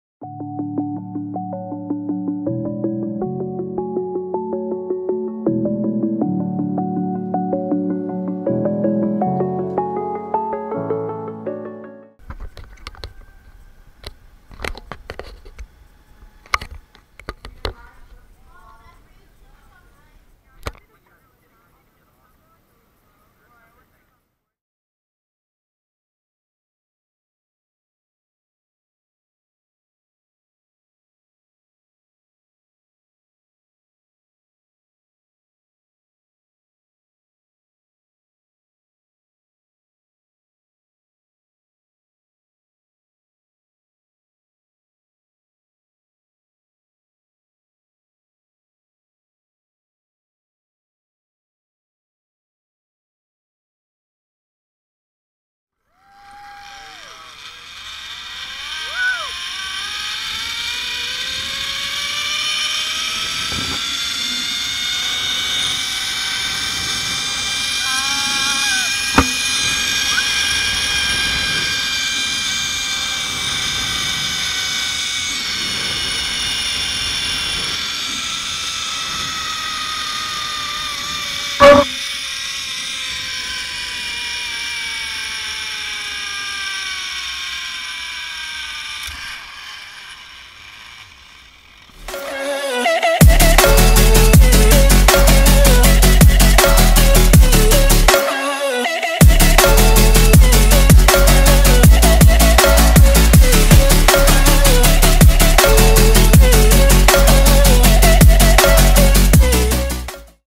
Zipline Adventure - Adventure Park (HD POV)
This is the "Zipline Adventure" zipline at Adventure Park in Wisconsin. The ride starts from a 50ft tower and riders zoom over a small pond to ground level.